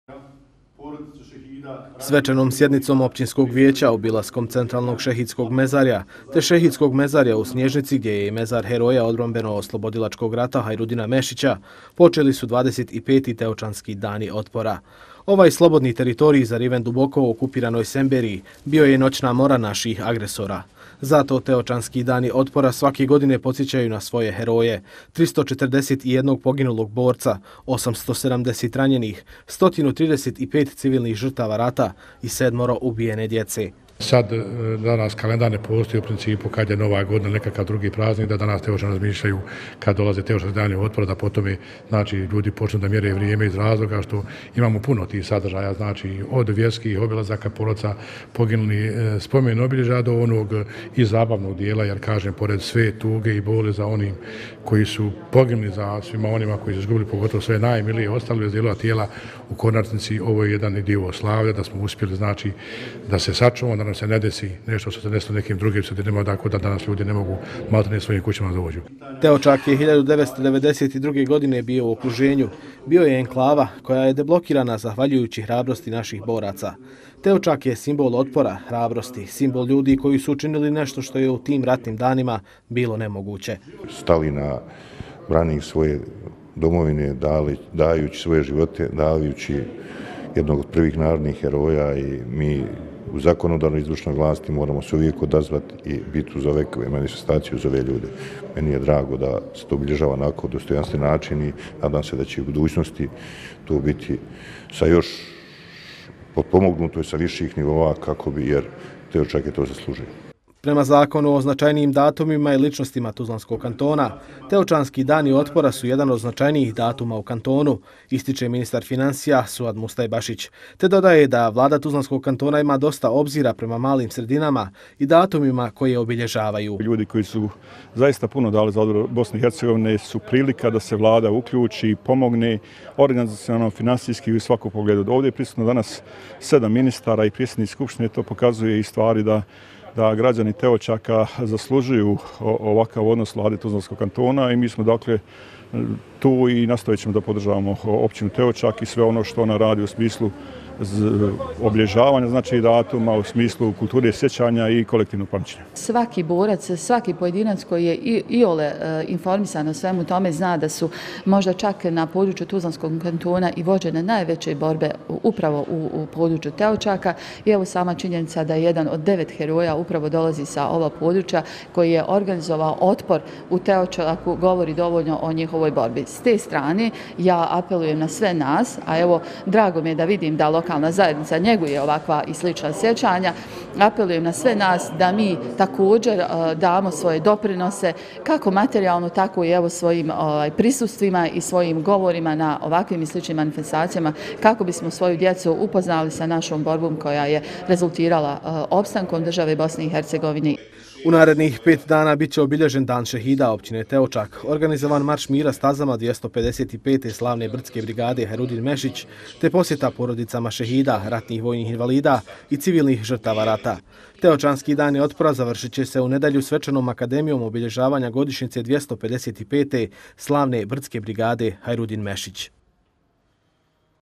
25. Teočanski dani otpora
U Teočaku su danas počeli 25. Teočanski dani otpora.  Ovom tradicionalnom manifestacijom Teočani njeguju sjećanja na svoje heroje, branitelje slobodne teritorije koja je, tokom rata u BiH, prkosila agresoru . U narednih pet  dana bit će obilježen Dan šehida općine  Teočak, organizovan Marš ratnim  stazama 255.Slavne brdske brigade “HajrudinMešić” te posjeta porodicama šehida, RVI i civilnih žrtava rata. Teočanski dani otpora završit će u nedelju, svečanom akademijom obilježavanja godišnjice 255.Slavne brdske brigade “HajrudinMešić”.